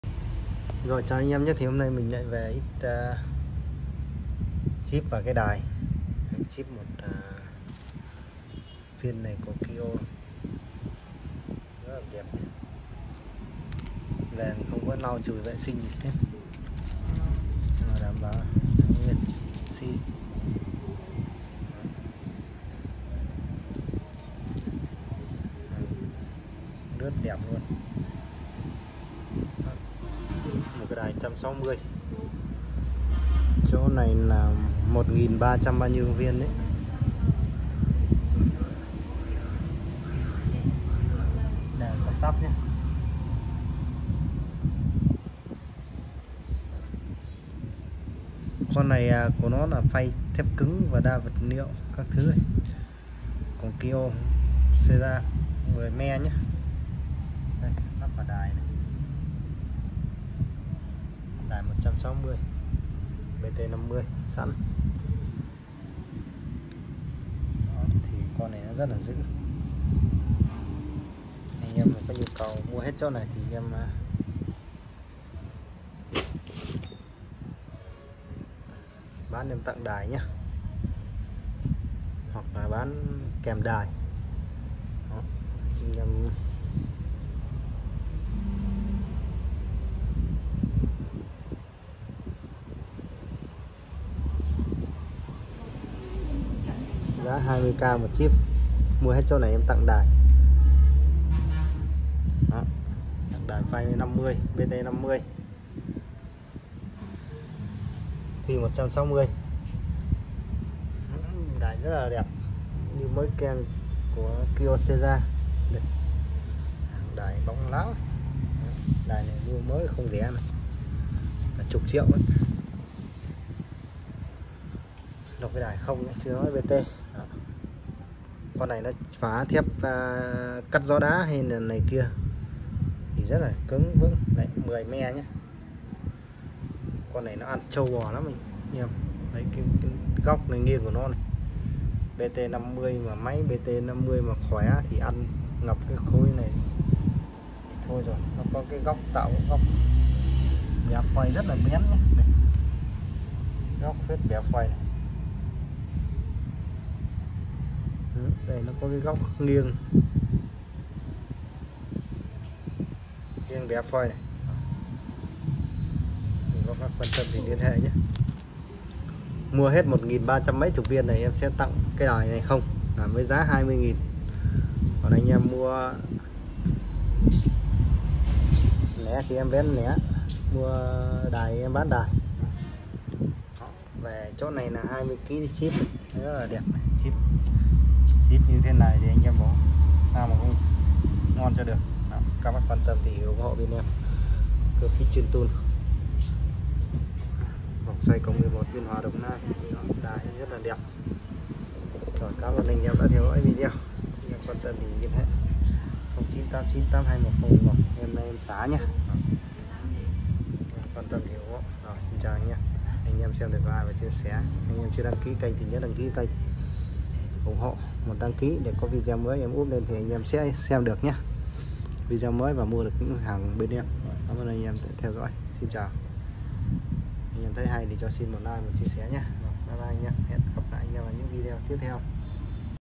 Chip kyocera ngũ giác . Chip 15mm 10 me cắt 2 mặt giá 20k tặng đài 160mm BT50 . Lh ngay 0989821011
Bán hết lô chíp tặng đài 160 Bt50 1360 chip siêu lướt . Anh em ủng hộ .